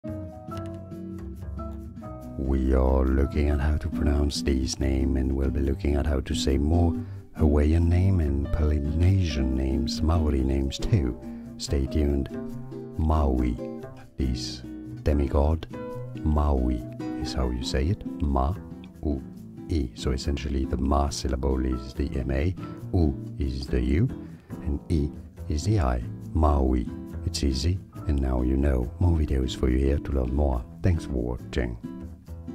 How to Pronounce Maui
This video shows you How to Pronounce Maui, pronunciation guide.
00:00 - Pronunciation Intro
00:05 - How to Pronounce Maui
00:20 - More Difficult Pronunciations